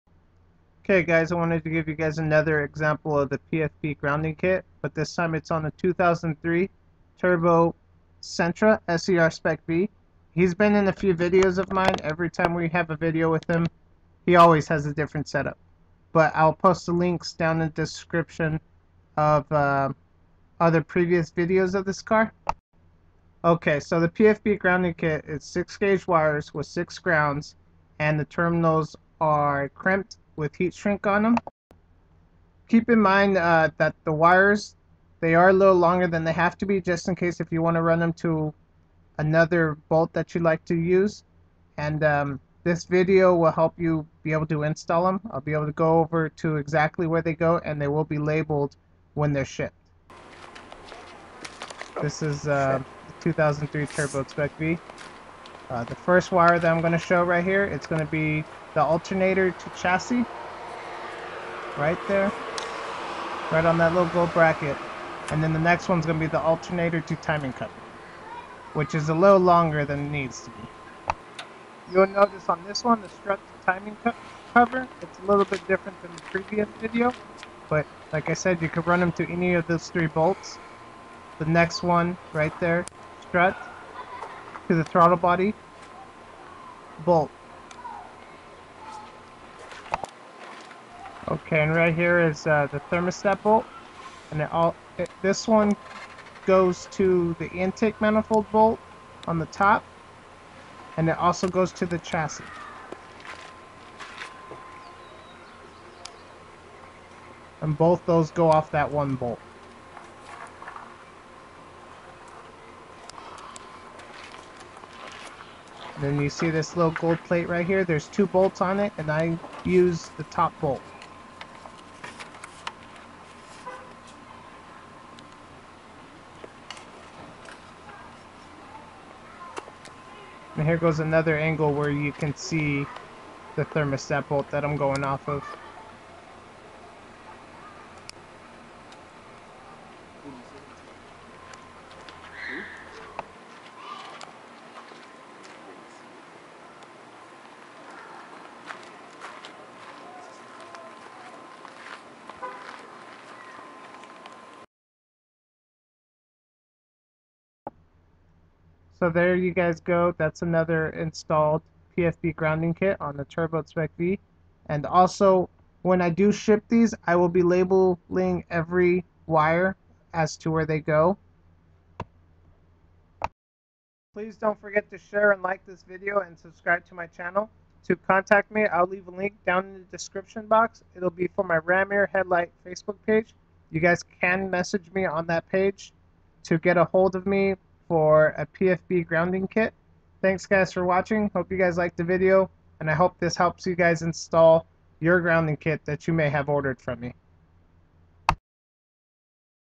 pFb Grounding Kit on 2003 Turbo Sentra Se R Spec V
pFb Grounding kit
2003 Turbo Sentra Se-R Spec V

6 Gauge wiring with 6 wires, kits will have crimped terminals with heat shrink.

This kit would be a direct bolt on to a 2002 to 2006 Sentra Se-R and Se-R Spec V.

1st alternator to chasis
2nd alternator to timing cover
3rd strut to timing cover
4th thermostat bolt to intake manifold
5th throttle body to strut
6th thermostat bolt to chasis

here are two other videos of this spec v.